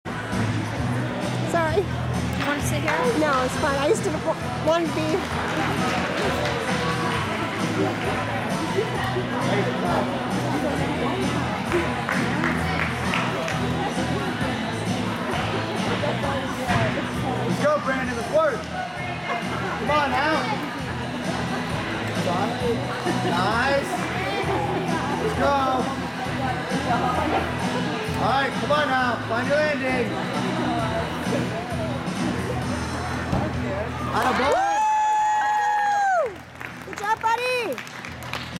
2022 Regional Championship (Region 1) - Level 5 High bar
This was not Brandon's best high bar routine this year but we are still proud of how far he's come this year! He scored a 9.05 for this one and got 9th place.